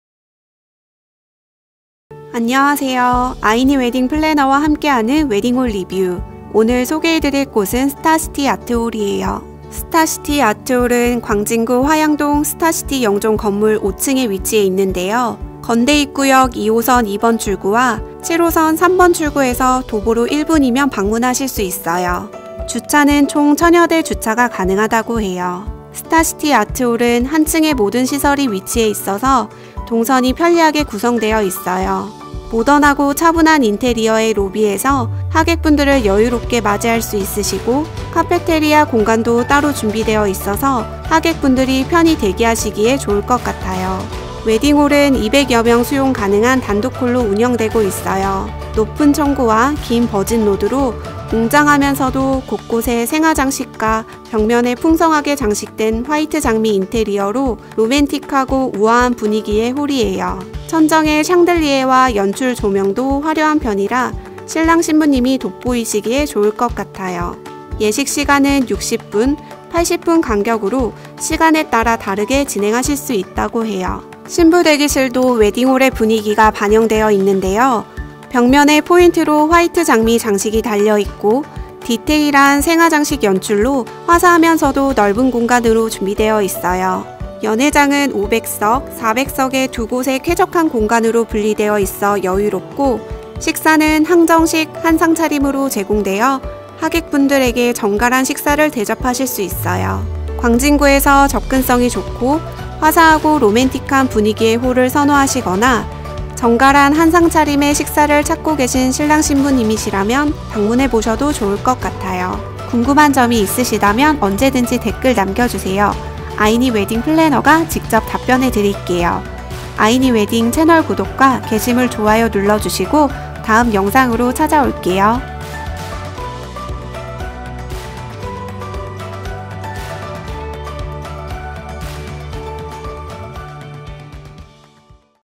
[ 광진구 건대입구 웨딩홀 ] 단독홀 , 한정식 맛집 , 로맨틱한 공간 스타시티아트홀 │리뷰 , 후기│ 아이니웨딩
안녕하세요 아이니웨딩입니다
아이니웨딩플래너와 함께하는 웨딩홀리뷰
소개해드릴 웨딩홀은 "스타시티" 입니다.

건대입구역 앞 스타시티영존 건물 5층에 위치해 있고
품격있는 공간 로맨틱한 분위기의 예식과
넓고 쾌적한 연회공간에서
맛좋은 한정식을 만나볼 수 있는 웨딩홀
지금 영상으로 확인해보세요!

궁금하신 점이 있으시면
댓글남겨주세요!
아이니웨딩 플래너가 직접 답변해드릴게요:)

웨딩홀 정보가 궁금하다면?

아이니웨딩에서 웨딩홀 전문가를 만나보세요